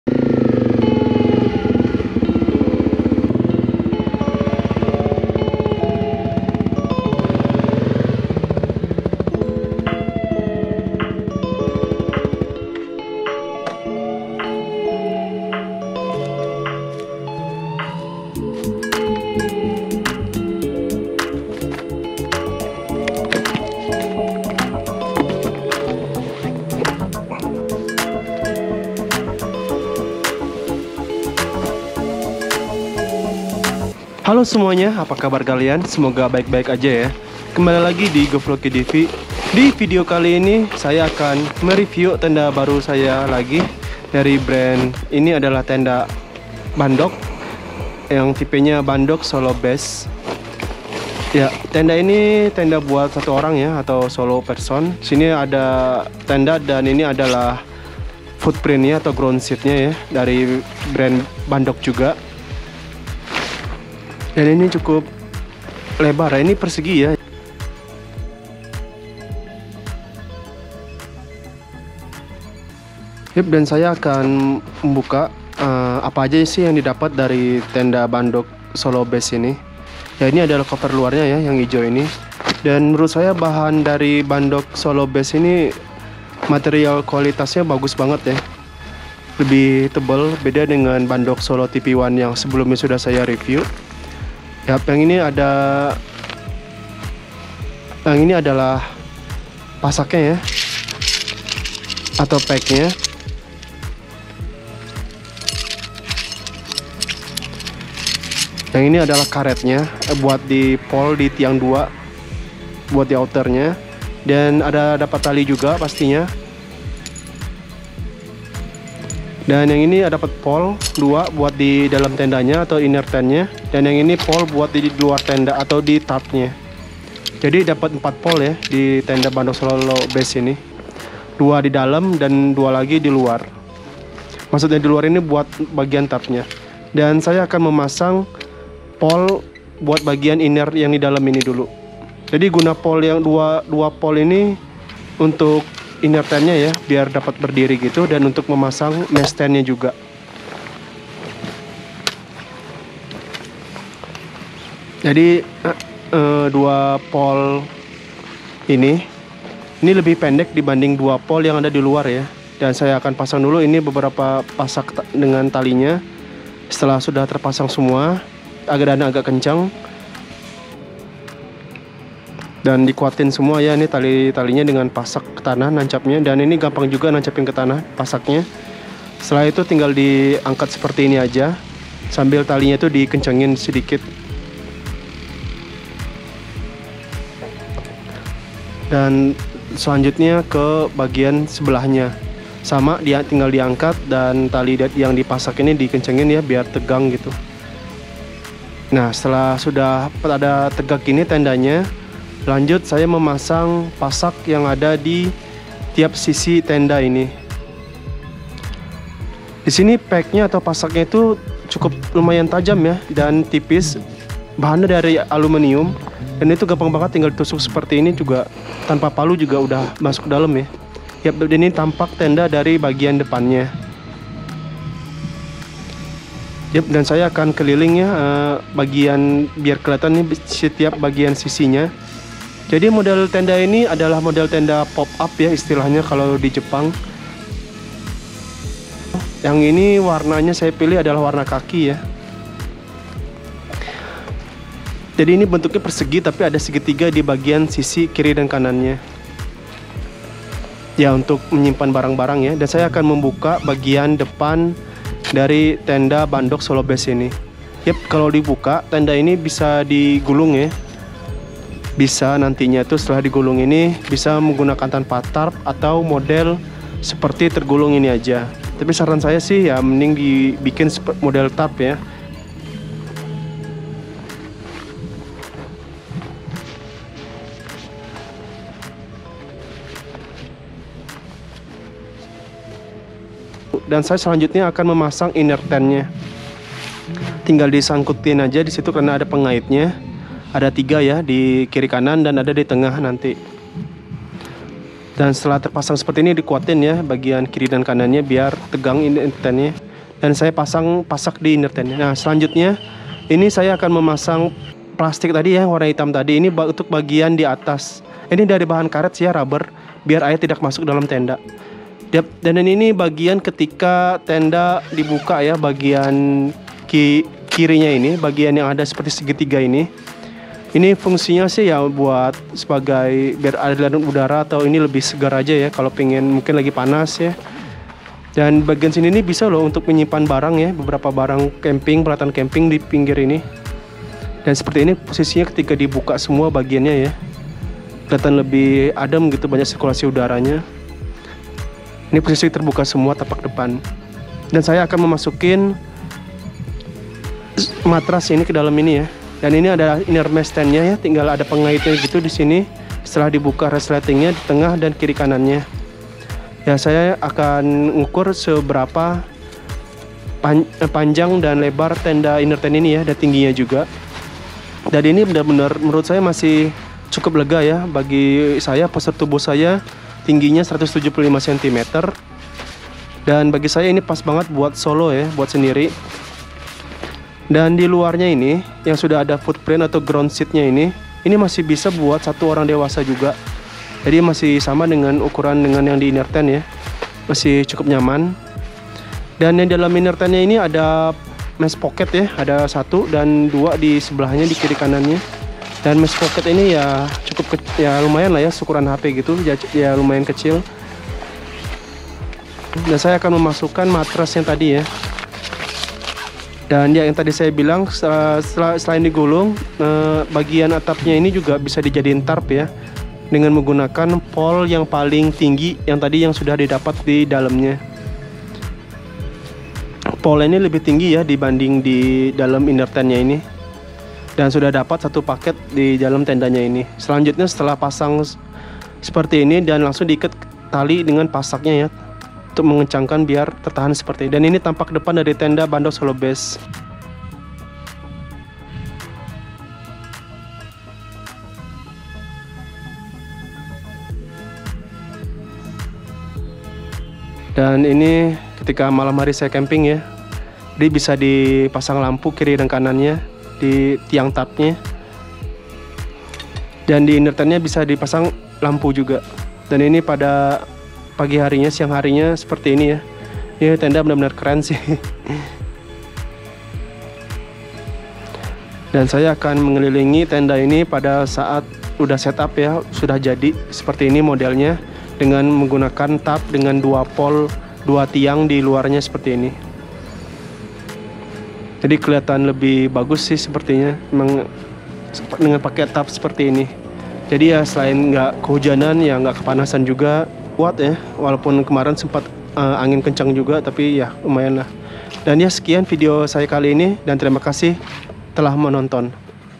REVIEW TENDA SOLO BARU! NEW TENT BUNDOK SOLO BASE BDK-79TC | バンドックソロベース | パップテント | 모토캠핑 |MOTOCAMPING
DI VIDEO INI SAYA MEREVIEW TENDA BARU YANG SAYA BELI SAAT DI JEPANG, YAITU TENDA BUNDOK SOLO BASE DAN BUNDOK GROUND SHEET. BUNDOK ADALAH BRAND CAMPING GEAR OUTDOOR YANG  BERASAL DARI JEPANG, YANG JUGA TERKENAL DI JEPANG NYA.

INI TENDA JENIS PAPTENT ATAU TENDA DENGAN 2 TIANG UTAMA UNTUK FLYSHEET DAN INNER MESH TENT NYA DAN 2 TIANG UNTUK TARP NYA, MODELNYA SEPERTI PERSEGI DENGAN SEGITIGA.

BAGI YANG BERMINAT DENGAN TENDA BUNDOK SOLO BASE INI, BISA PREORDER (PO) MELALUI SAYA DENGAN DM DI INSTAGRAM SAYA
TENDA INI DI IMPORT LANGSUNG DARI JEPANG. ESTIMASI NYAMPE INDONESIA NYA PALING LAMA SEKITAR 2-3MINGGU, TAPI BIASANYA LEBIH CEPAT, KURANG DARI 2MINGGU KALO SHIPPING NYA LANCAR.

HONDA CRF250RALLYでソロキャンプ。CRF250RALLYで旅をしてみました。キャンプだ！久しぶりのソロを満喫したらゆるすぎた。今回はBUNDOKバンドックソロベースBDK-79TCです。BUNDOKのパップテント。

▲TENT / テント
BUNDOK SOLO BASE
バンドックソロベースBDK-79TC

久し振りにテントを買いました。コンパクトさにかけてはモノフレームシェルターで十分なので、今回は快適性を重視して、この商品を選びました。
長い事品切れで、やっと出たのが、たまたまアマゾン限定のこの製品。良く分からずに買ったら、珍しい色だったみたいです。
簡単な設営、シンプルな構造、広い空間、お手軽な価格…あまりこだわりのない私でも魅力的に感じて購入に至りました。
詳細はメーカーページの方が詳しいですし、カッチリしたレビューでもなく、数字的な部分はほどほどに、見て感じる部分を中心に編集しました。何かの参考になれば幸いです。

HUGE THANKS TO ALL OF YOU FOR FOLLOWING OR SUBSCRIBER AND SUPPORTING MY ADVENTURE ALONG THE WAY! LIVE PASSIONATELY. FOLLOW YOUR DREAMS.